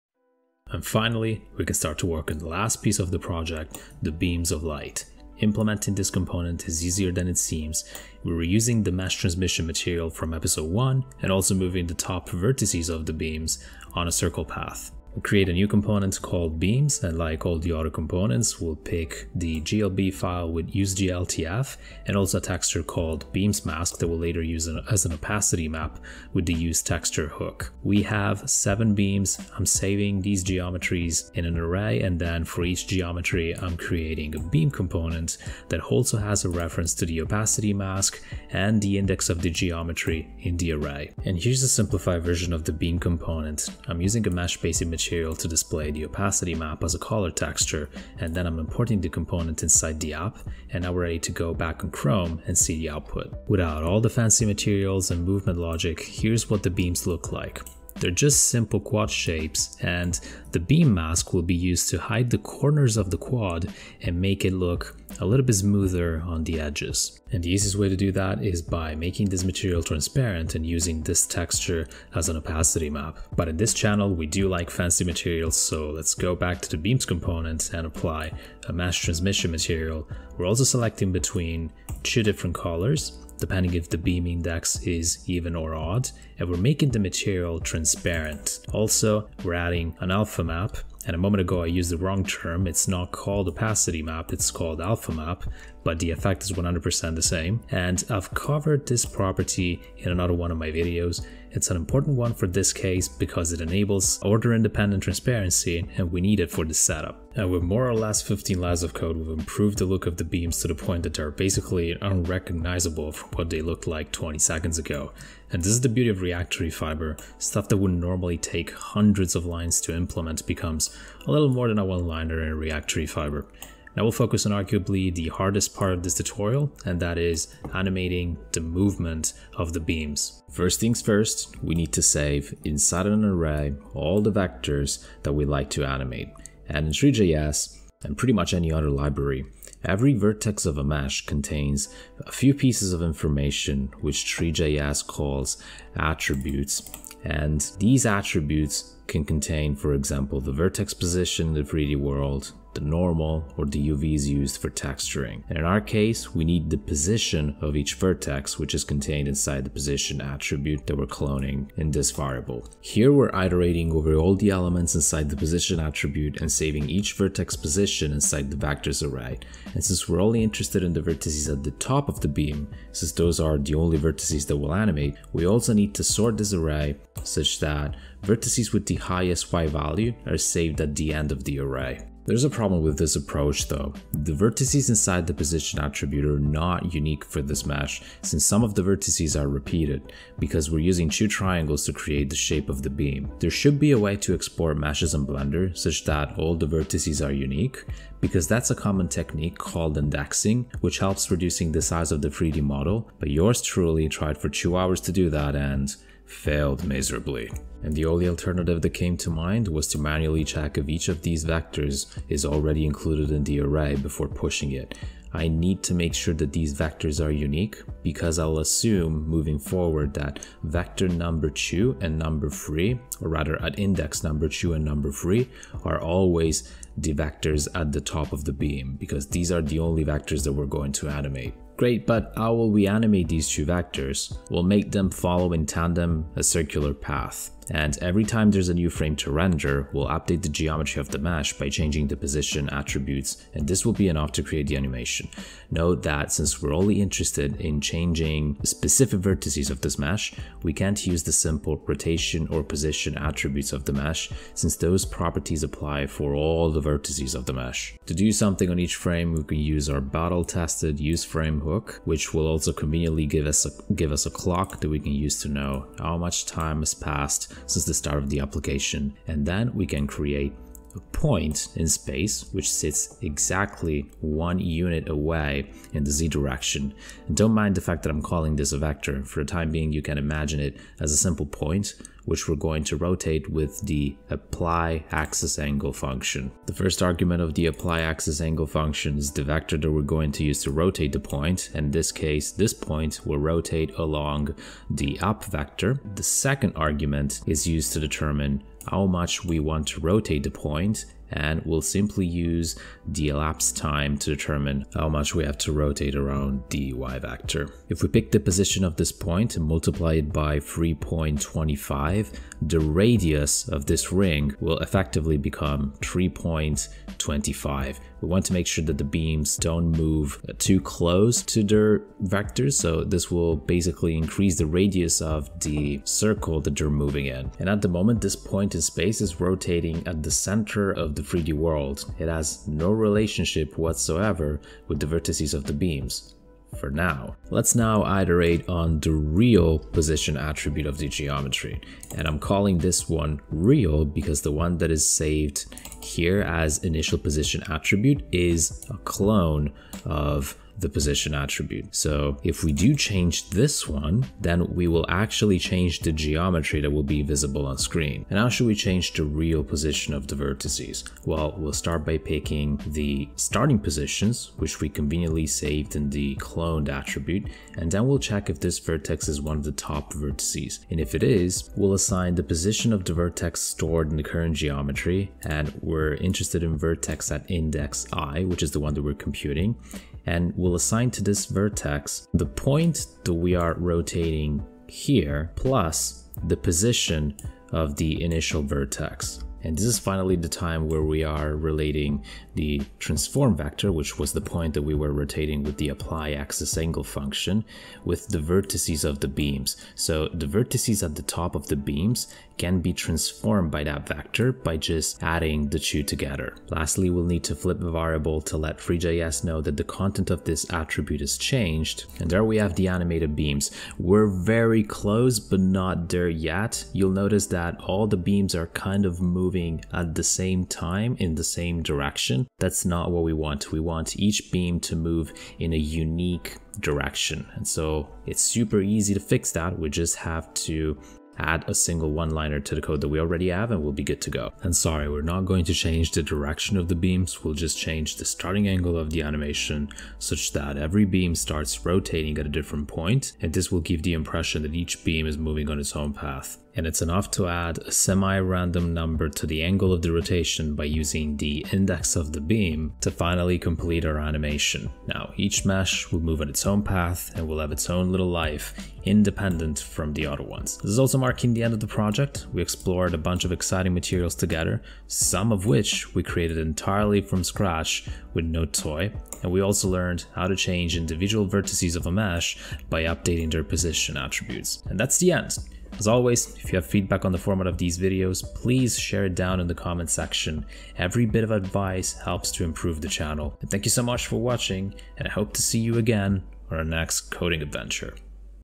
Custom shaders w/ React Three Fiber and Nodetoy -Meteor Impact- Tutorial ep 4
And now it's time to complete our journey by animating the beams of light, we'll learn how to manipulate threejs' geometry attributes and how to update the position of specific vertices of a mesh, while keeping the other vertices unchanged. This little trick will be the only thing that's necessary to create an interesting animation and it'll mark the end of this project

Hope you guys had fun on this one! We learned a lot of interesting topics togheter, and we started to finally introduce shaders on this channel, which will open up a lot of future possibilities for upcoming videos, stay tuned!

Try it on your browser!

Intro: 0:00
Simplified beam component: 0:15
Transmissive material: 1:13
Beams animation setup: 2:10
Animation logic: 3:50
Random rotation offset: 7:14
Conclusion: 7:39